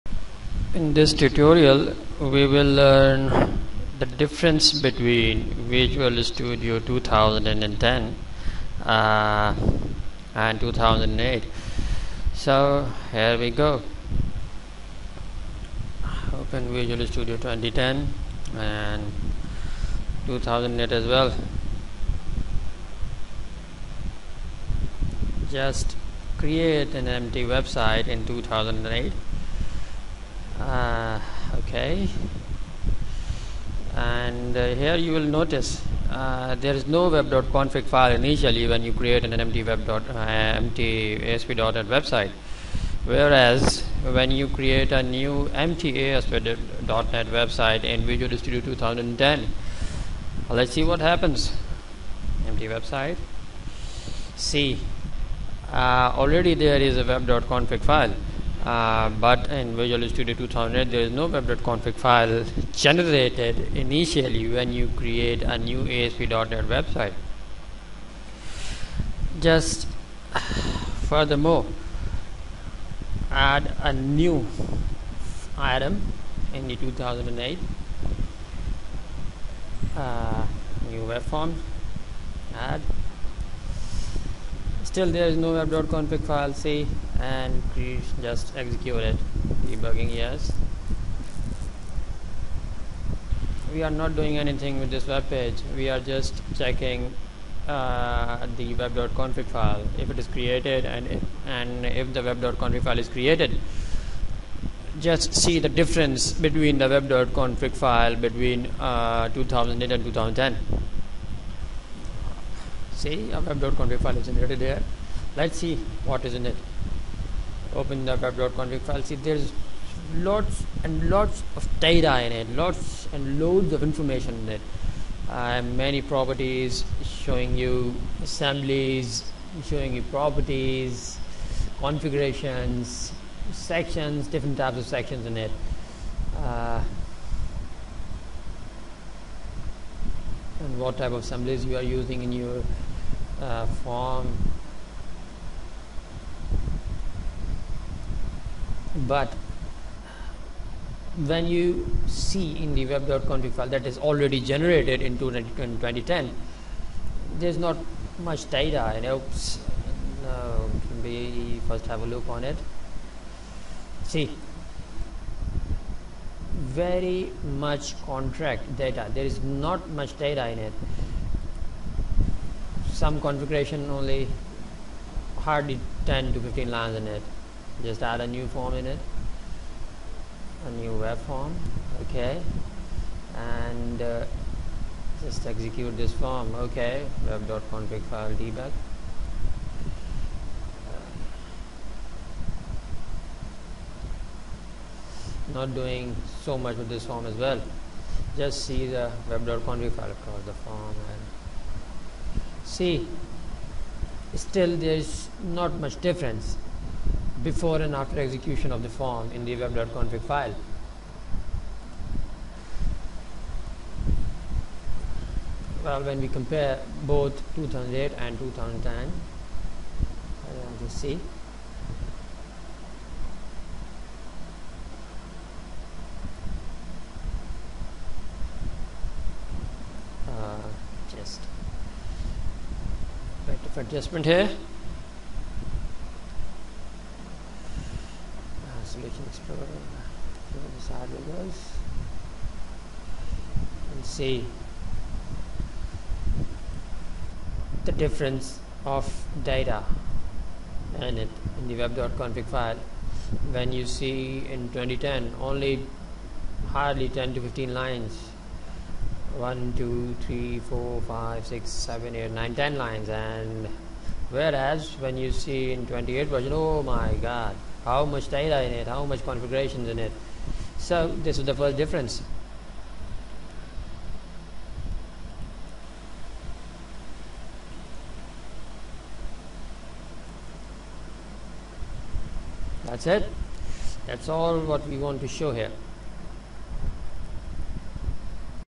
Difference Between VS2010 and VS2008 in Web.config file
In VisualStudio 2008 we have lots of  configuration settings in the web.config file,means if you create a simple  empty asp.net website you will notice so many properties in the web.config file where as when you see VisualStudio 2010 there is not much properties in the web.config file